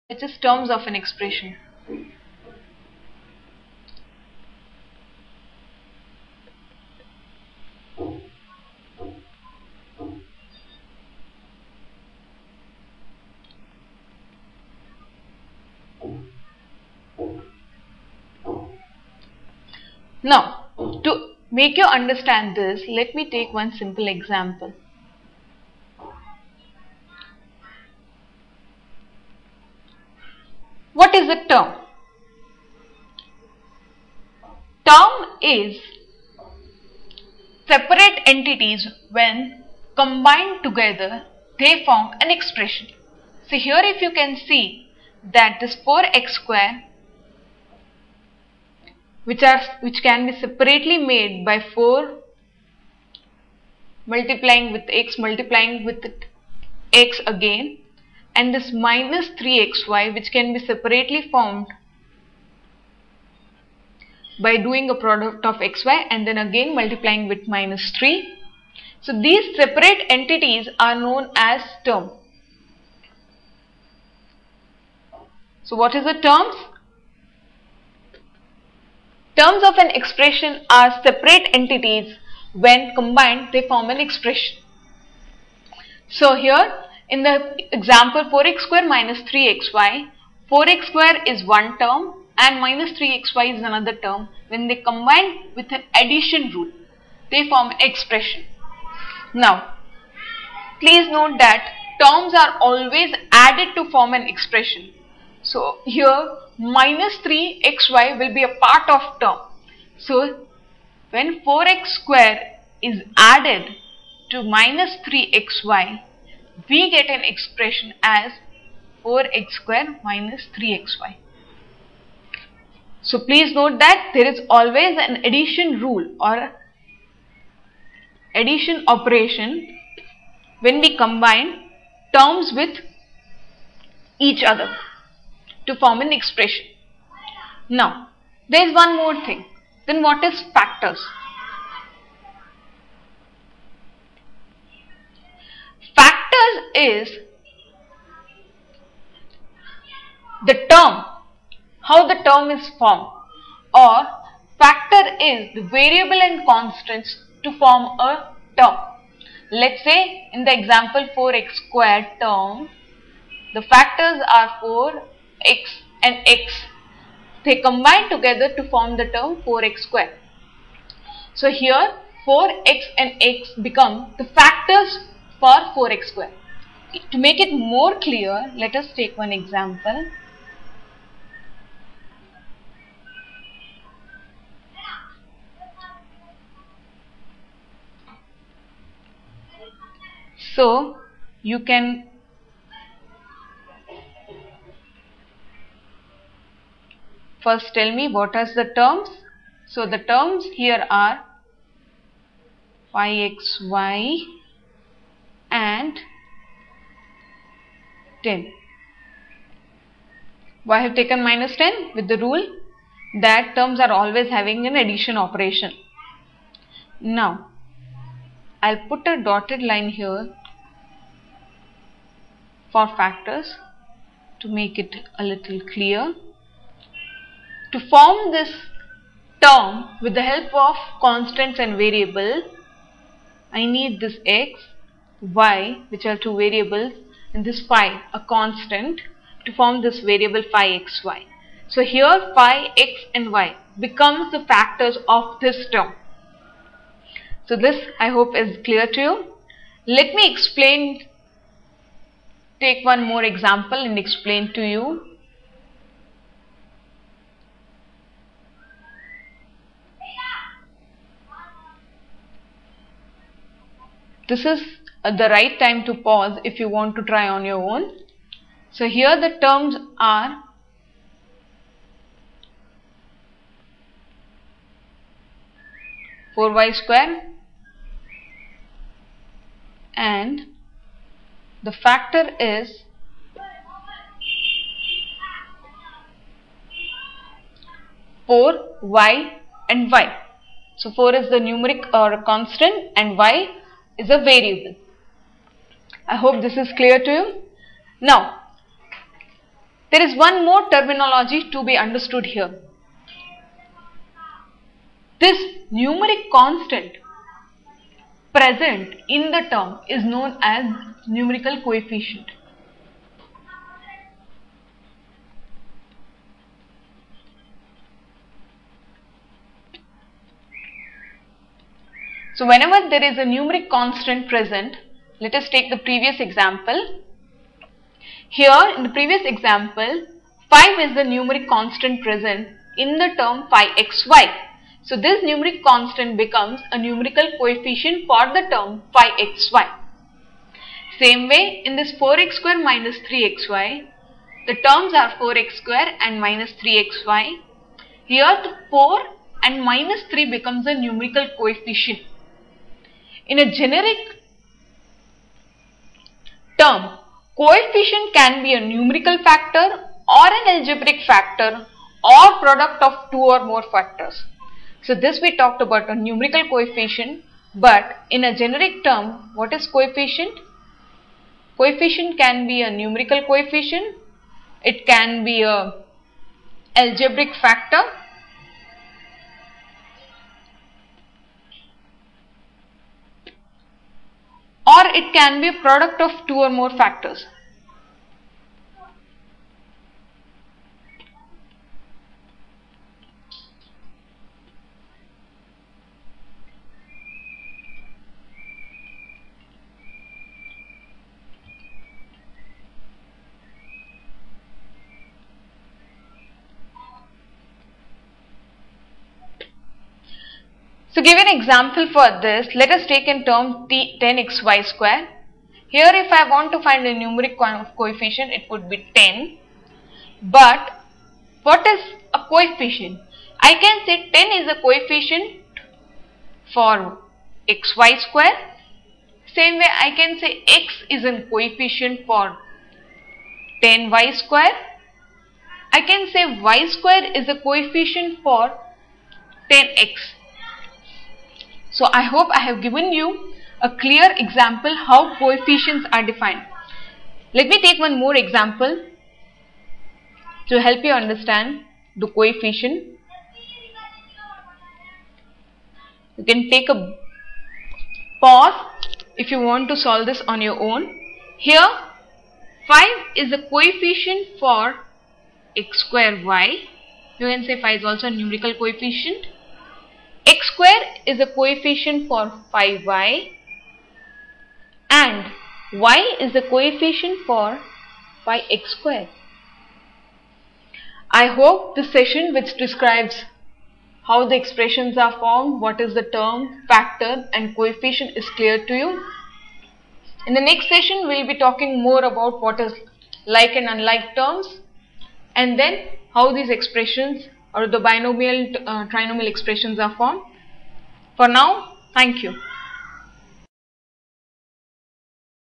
02_Terms factors and coefficient in Algebraic expressions. CBSE MATHS
NCERT Syllabus Std 7 Chapter 12 Algebraic Expressions. This video discusses - Terminology of Algebraic expressions like terms factors and numerical coefficient.